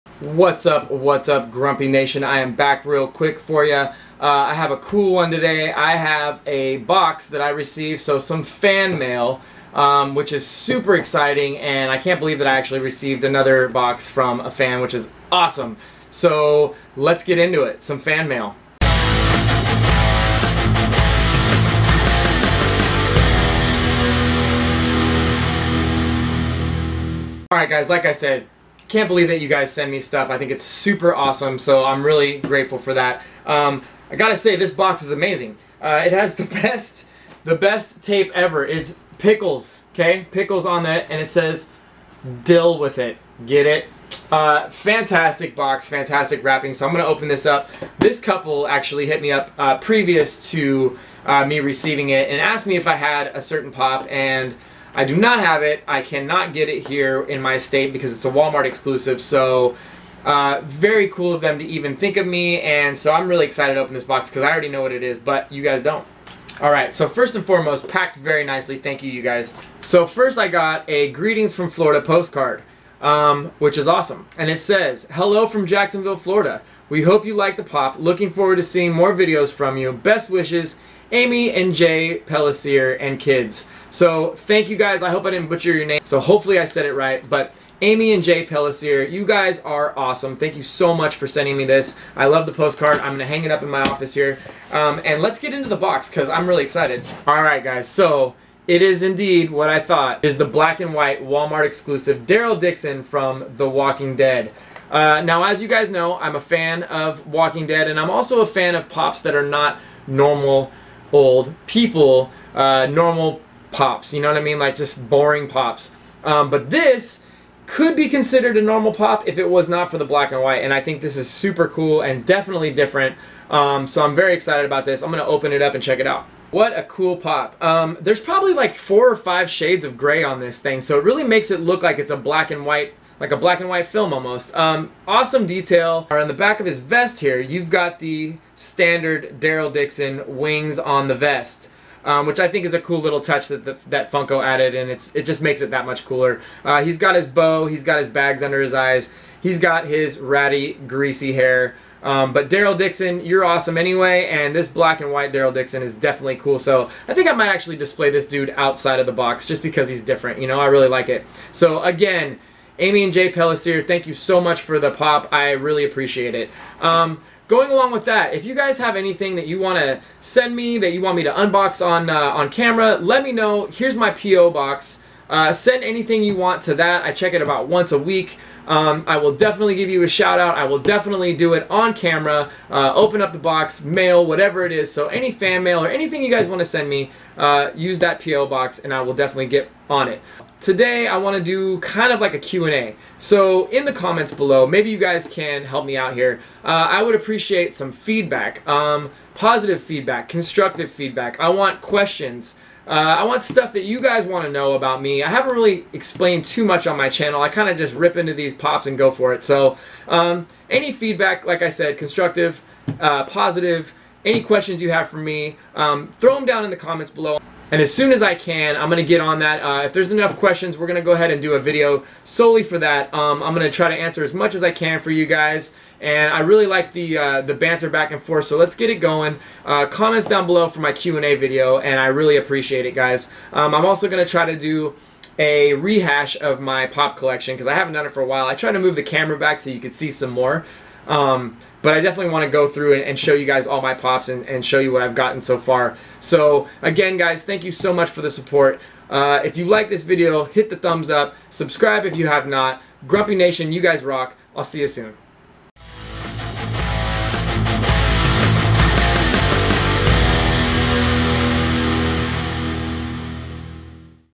Fan Mail :: Q&A Sesh'
Today I unbox an Awesome piece of Fan Mail!! Also I throw out some ideas for y'all about the upcoming Q&A session. Hope to hear from all of you out there in GrumpyNation!!!

Want me to unbox something from you guys? Send it here!

The Grumpy Nerd
Independence, Oregon 97351